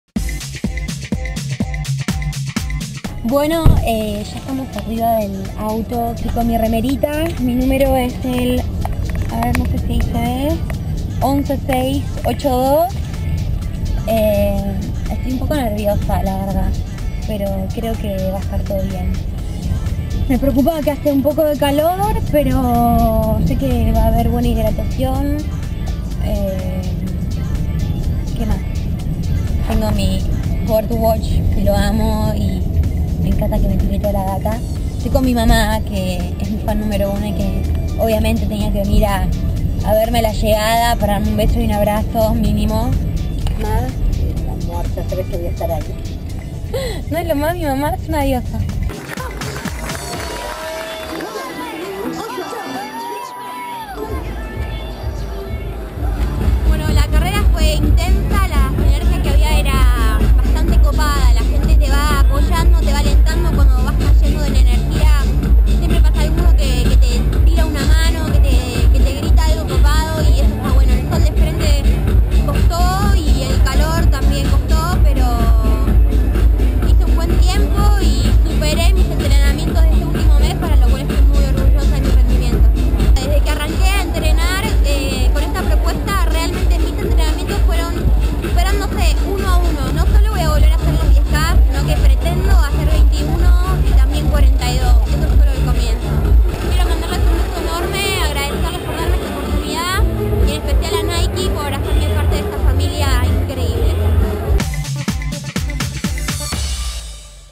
Jenny Williams - Nike 10K
Experiencia Nike 10K werunbue 2012 contada en primera persona por Jenny Williams